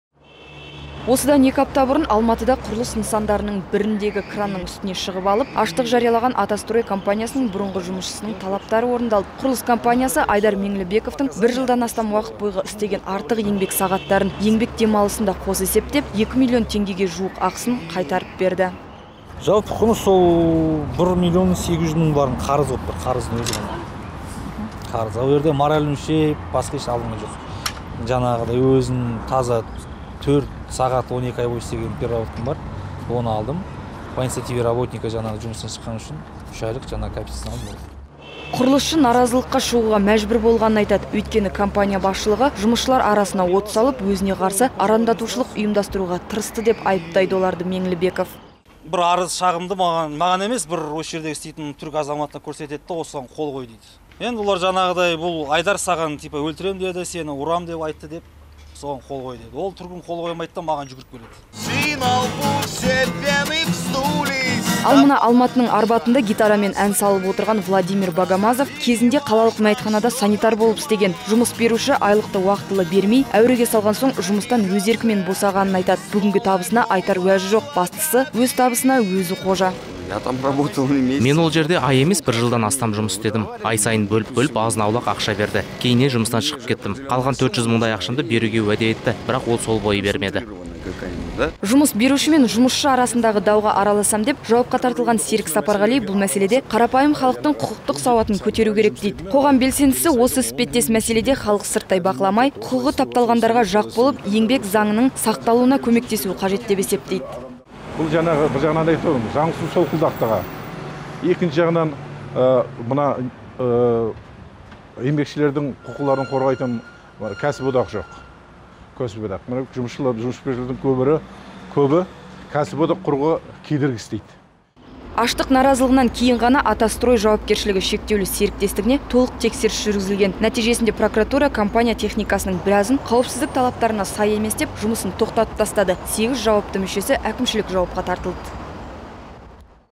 Құрлысшы өз құқығын қорғады
Алматыда Екі апта бұрын құрылс басындағы кранның  үстіне шығып алып аштық жариялап, наразылығын білдірген «А-та стой» ЖШС бұрынғы  жұмысшысының талаптары орындалды. Аталмыш  құрлыс компаниясы  Айдар Меңлібековтың, бір жылдан астам уақыт бойғы, жұмыс істеген артық сағаттарын,еңбек демалысын қоса есептеп 2мллн теңгедей ақшасын қайтарып берді.